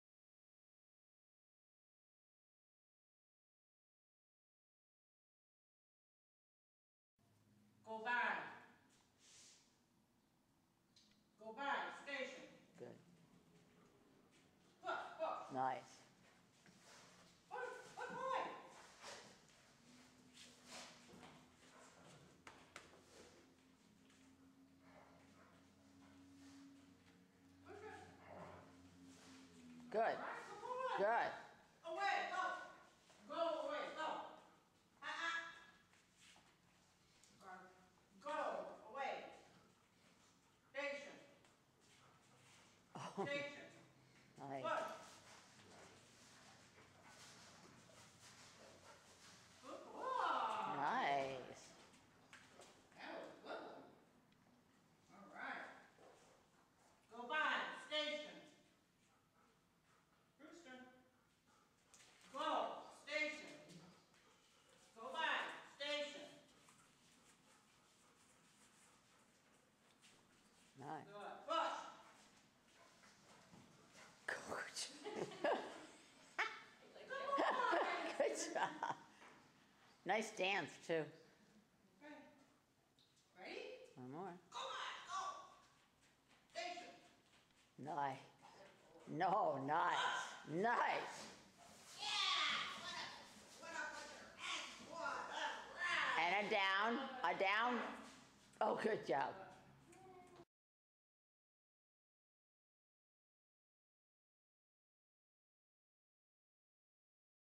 Brewster - Treibball
Brewster and Yvonne at Treibball Class
DogTrainingByBonita.com
Communicate & Play Dog Training